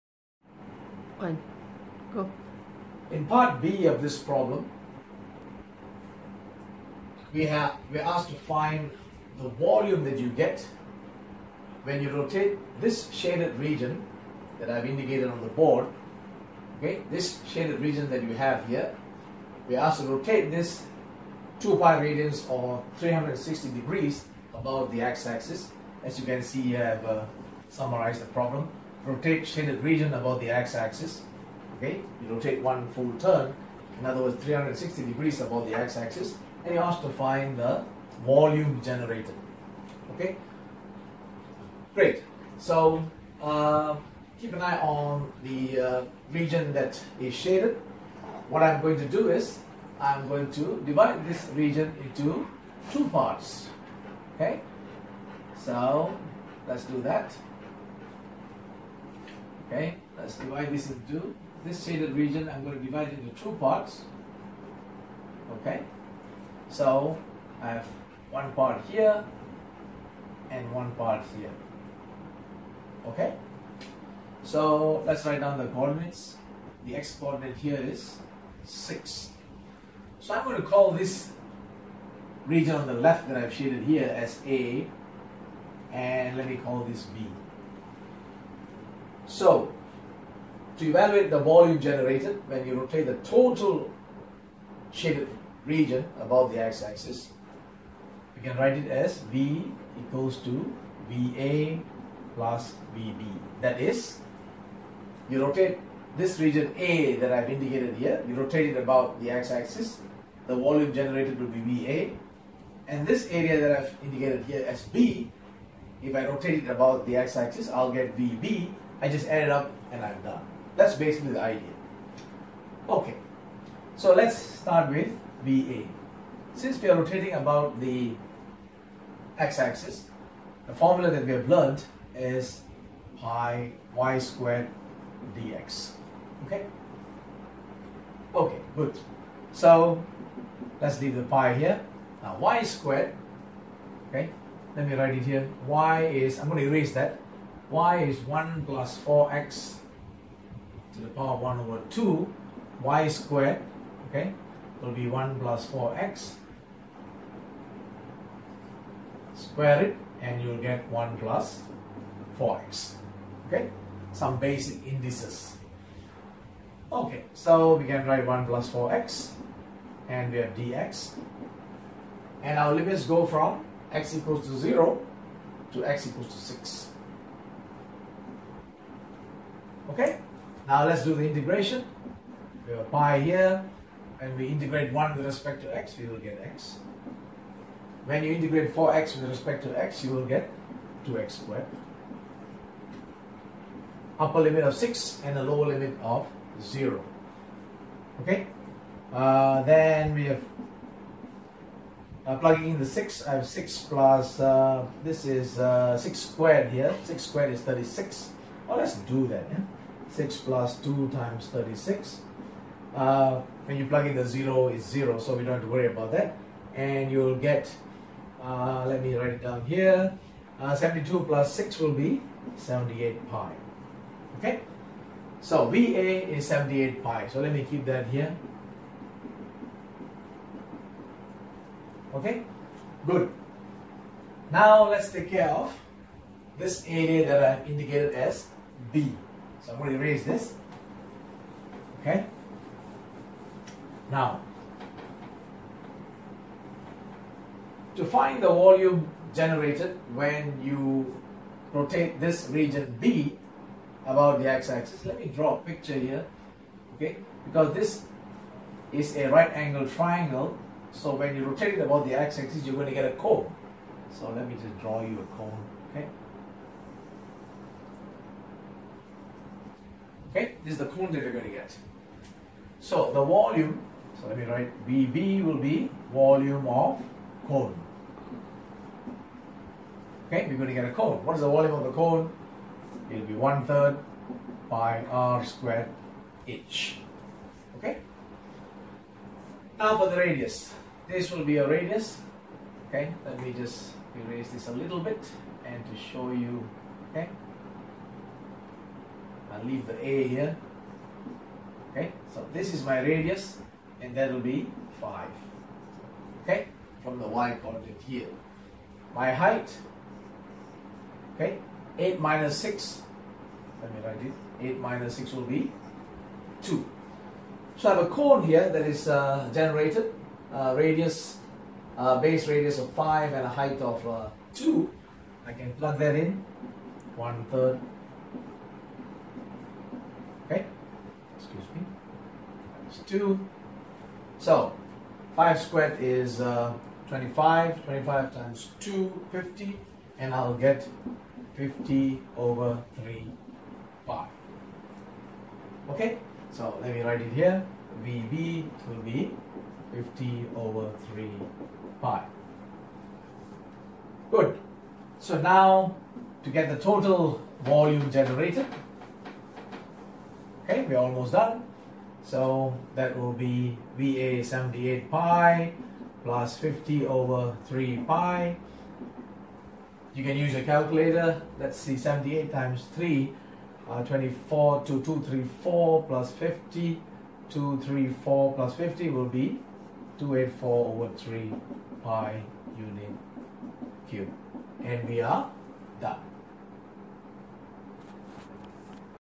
CIE AS P1 Maths 9709/11 Oct/Nov 2015 q11 part B full solutions
Cambridge AS Levels Pure Maths 1 (P1) 9709/11 Oct/Nov 2015 Question 11 part B Exam Solutions
‪#‎CIE‬ ‪#‎Maths‬ ‪#‎PureMaths‬ ‪#‎ExamSolutions‬ ‪#‎Cambridge‬